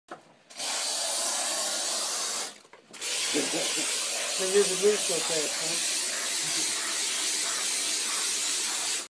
Spraying a flash coat of foam on a tongue and groove ceiling to air seal it
Tongue and groove wood is extremely difficult to air seal well. The only good solution is 2 part polyurethane foam. Note that the edges were already sprayed before the flash coat. It's very important to span the corner of the cavity, if you don't hit both sides at once the foam is very prone to pulling away from one side. This is a smallish 600 kit of foam which does about 600 board feet, or 600 square feet 1 inch thick.